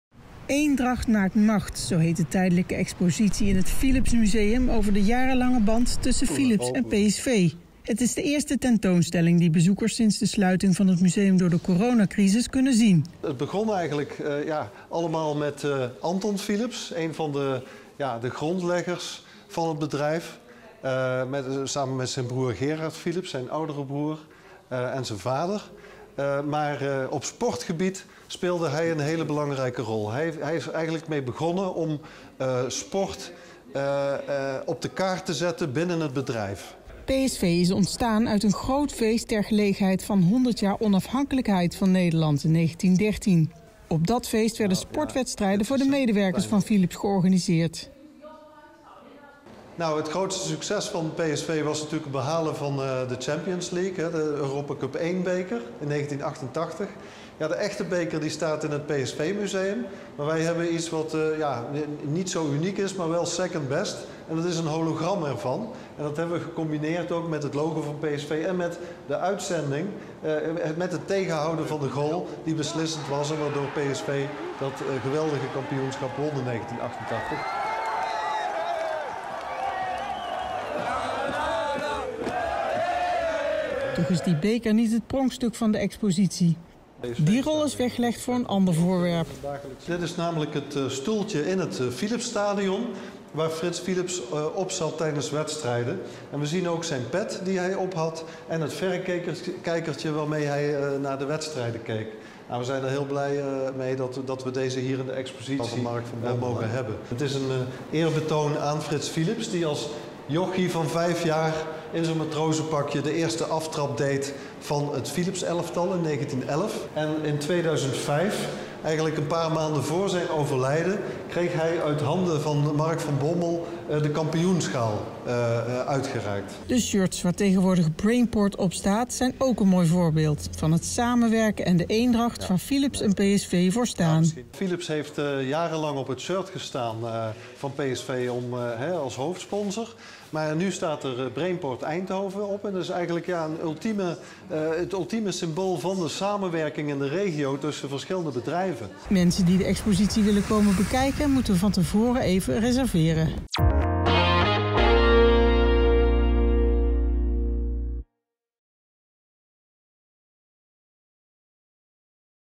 Expo over de sterke band tussen Philips en PSV
Eendracht maakt macht, zo heet een tijdelijke expositie in het Philips museum over de jarenlange band tussen Philips en PSV.